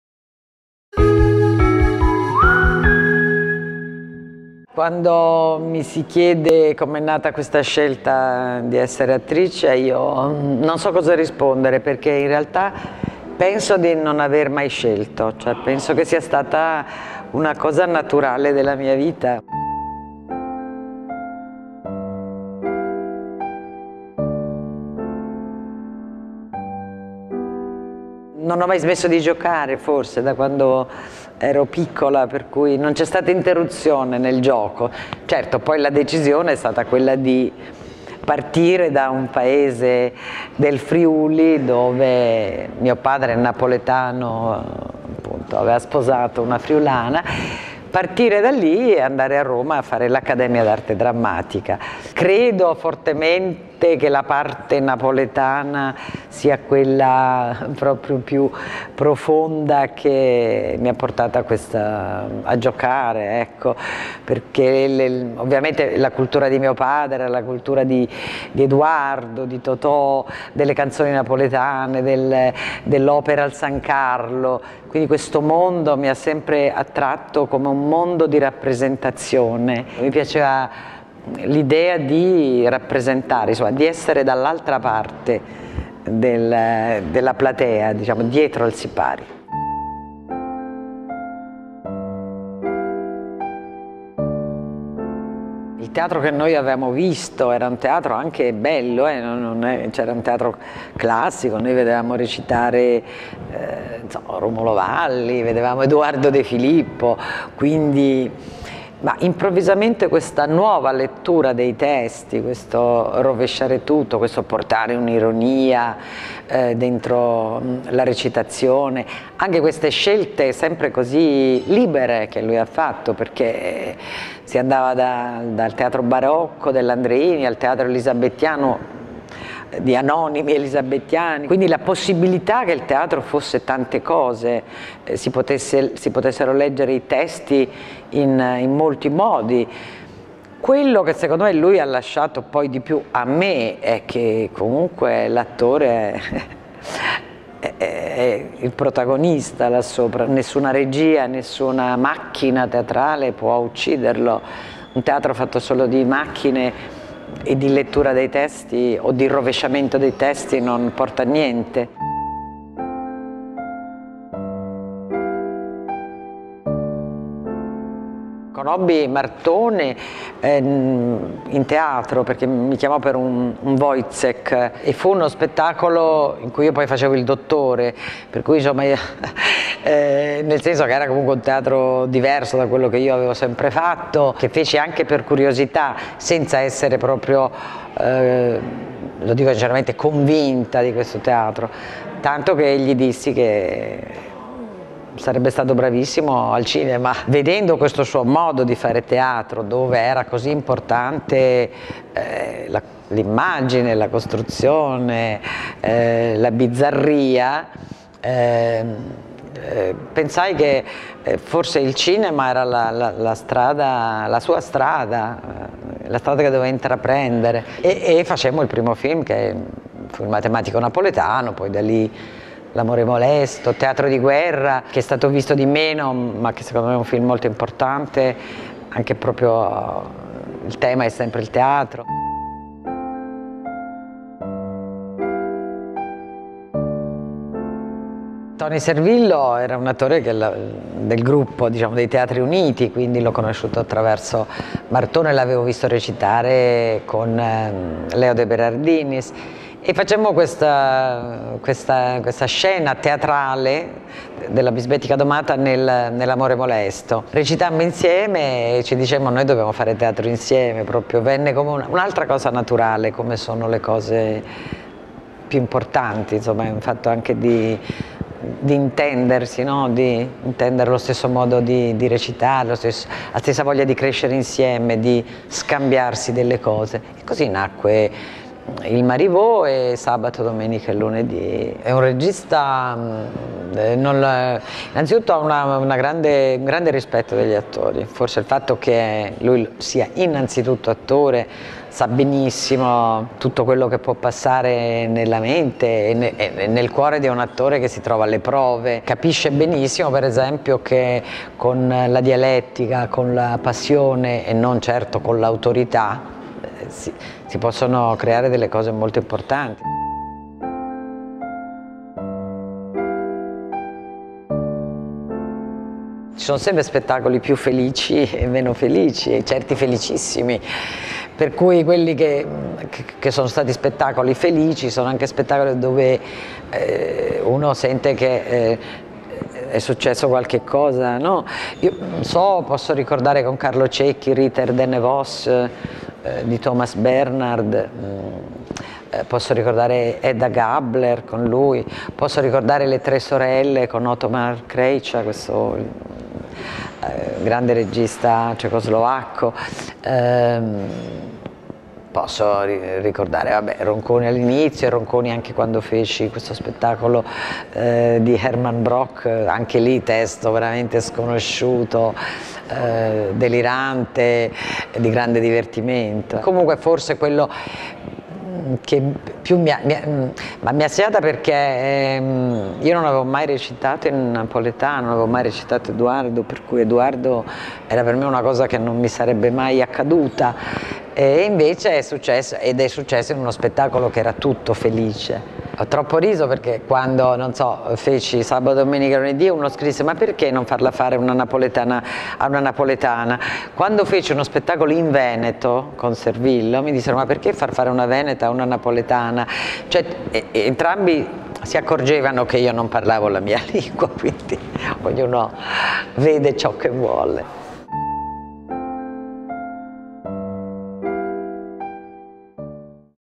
Anna Bonaiuto: "Non ho mai smesso di giocare"
È la signora del teatro italiano, antidiva per eccellenza. Ripercorriamo insieme a lei oltre trent'anni di carriera dagli esordi con Luca Ronconi, ai lavori con Carlo Cecchi e Mario Martone, fino all’indimenticabile interpretazione di Donna Rosa Priore in “Sabato, domenica e lunedì” di Toni Servillo.